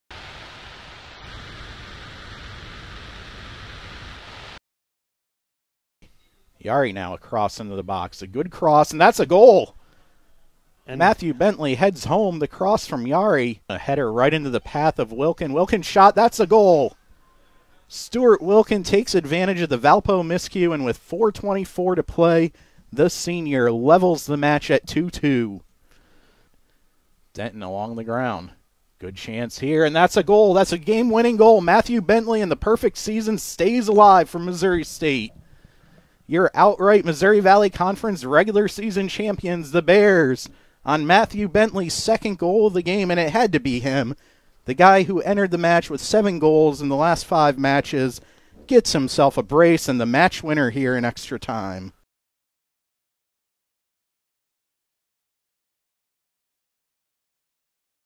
Top 10 MSU Moments of the Year: #7 - Late Comeback Clinches MVC Title for Men's Soccer
Three unanswered goals secured the MVC title for MSU Men's Soccer in an overtime thriller at Valpo.  Matt Bentley scored a pair of goals, including the game-winner, and Stu Wilkin delivered the equalizer with a goal in the closing minutes to send the match to OT.